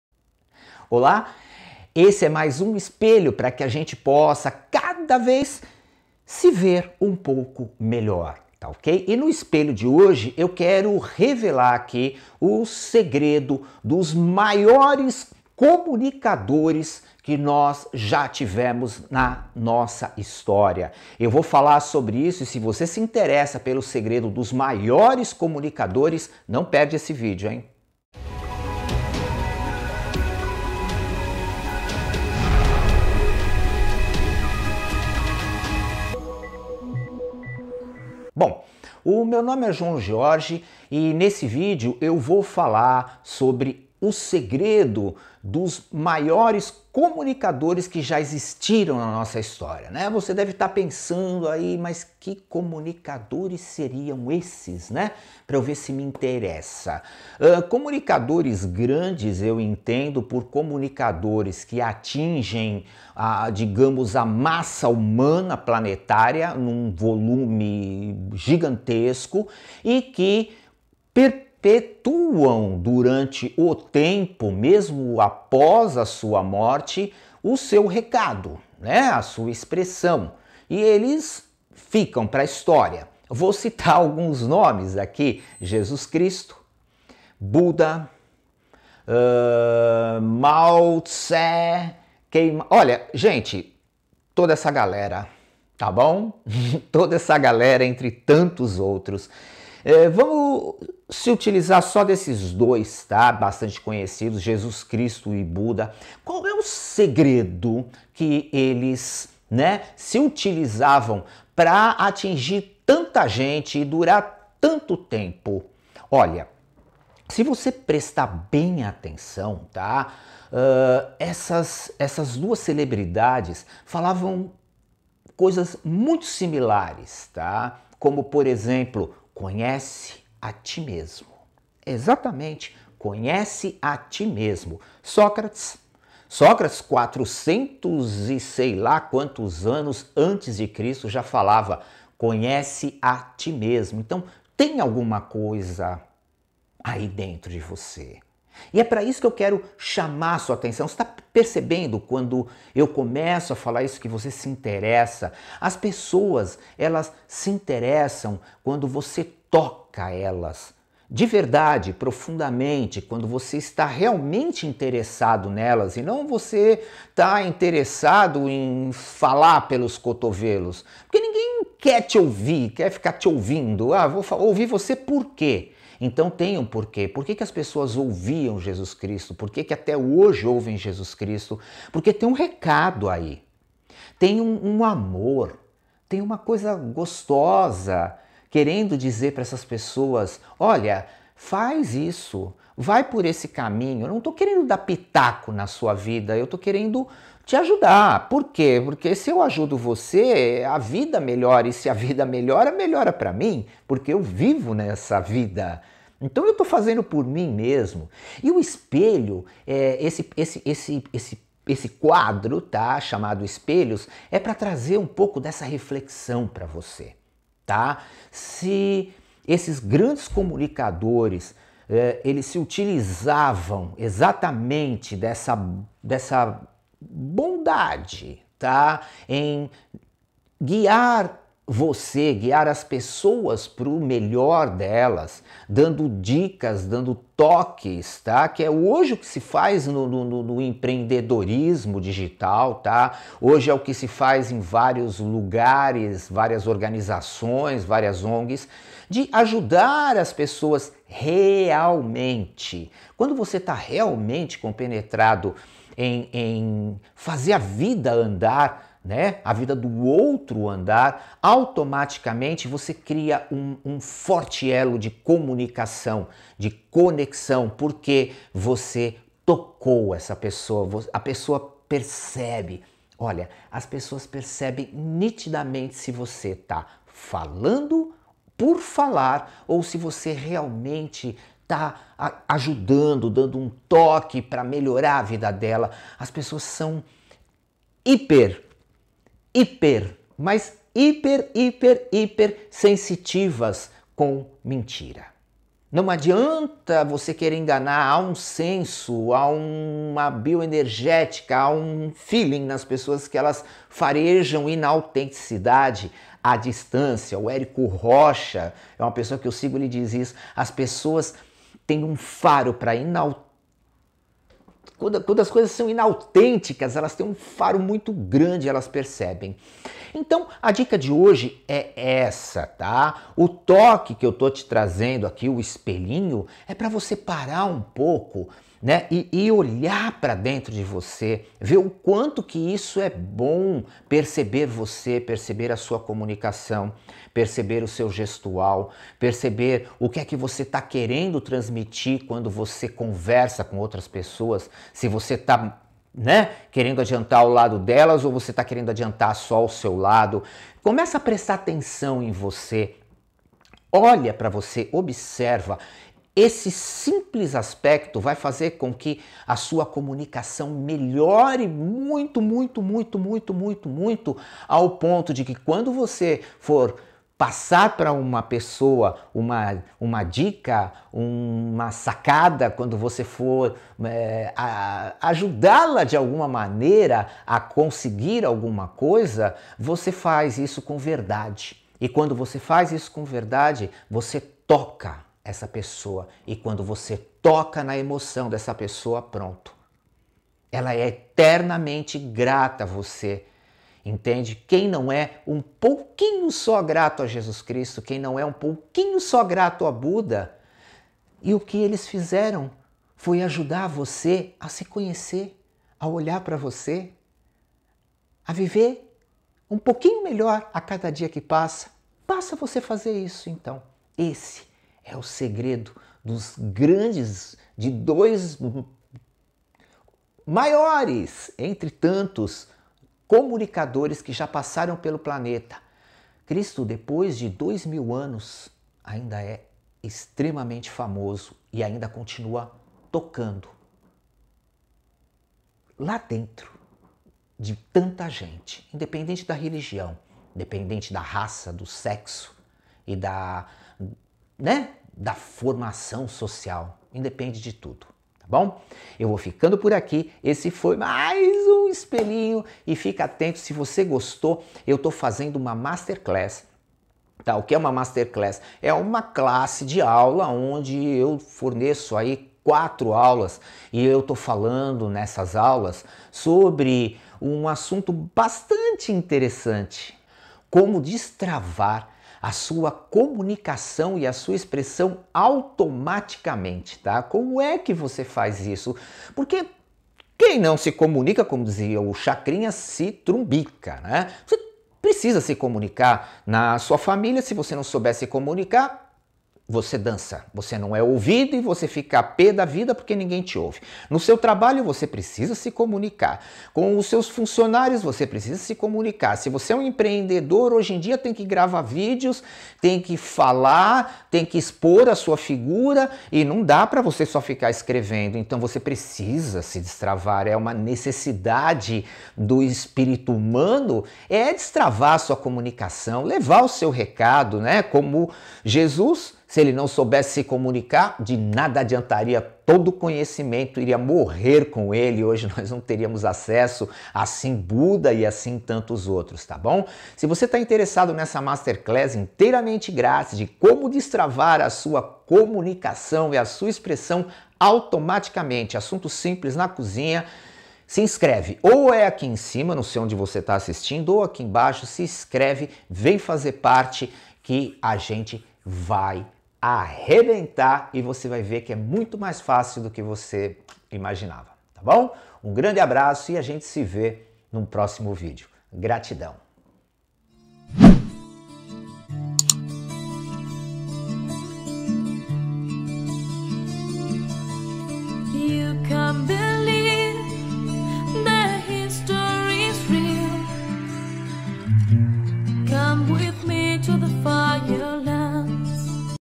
O Segredo dos Maiores Comunicadores do Mundo | João Gyorgy
Nesse video eu revelo o grande segredo usado pelos maiores comunicadores do mundo... o que fez com que eles continuassem famosos, assim como o seu recado, através do tempo. Imperdível.

Você também pode me encontrar ↴